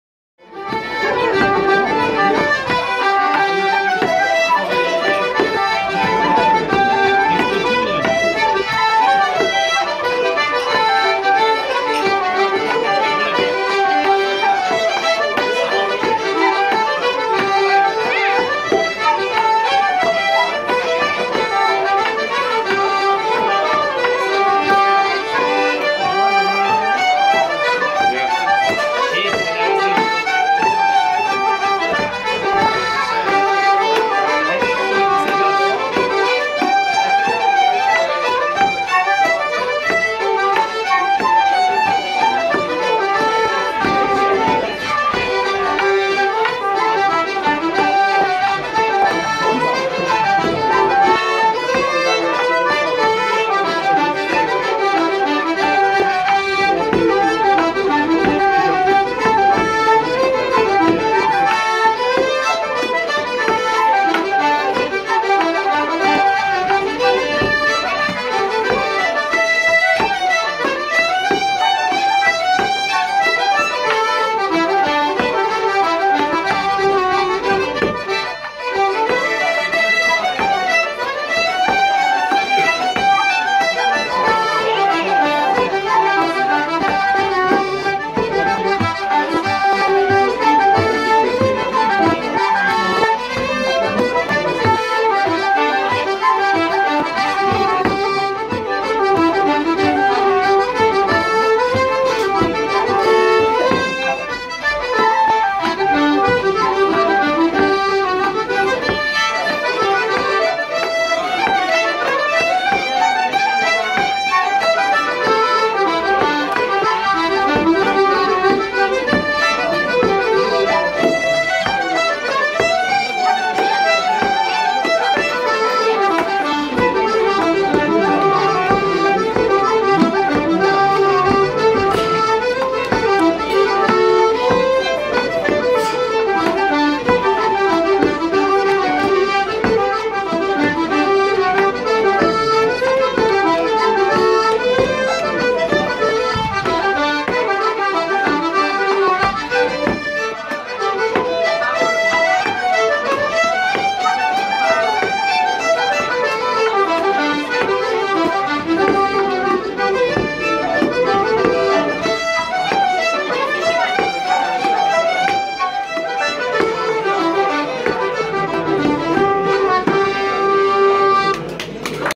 Session in Doolin O'Connor's Pub in Doolin with Christy Barry and Terry Binghman
Had a great session in Doolin in O'Connor's Pub and playing some hornpipe